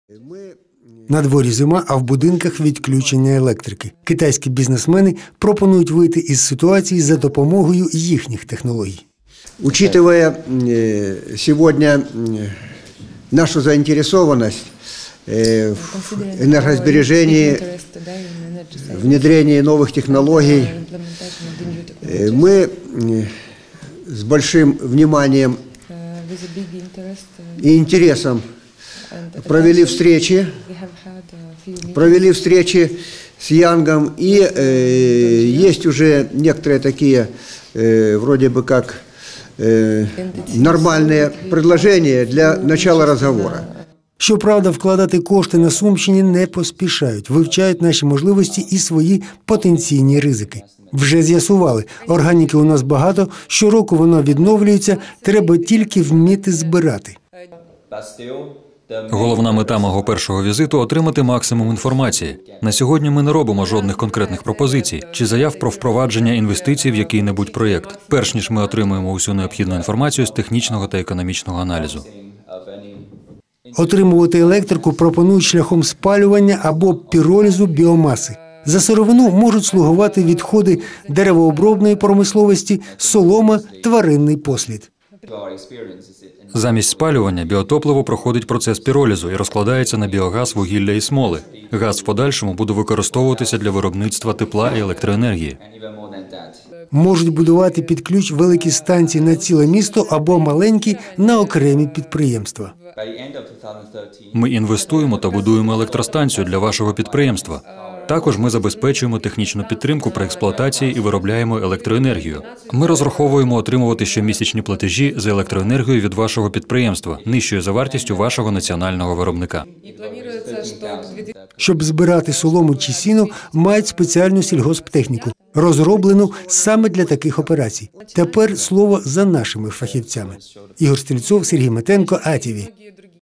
Китайський енергетичний досвід
Китайський енергетичний досвід. Сумському бізнесу та посадовцям презентували виробничі можливості та інвестиційні пропозиції однієї з китайських компаній, яка спеціалізується на технологіях з видобутку електроенергії і тепла.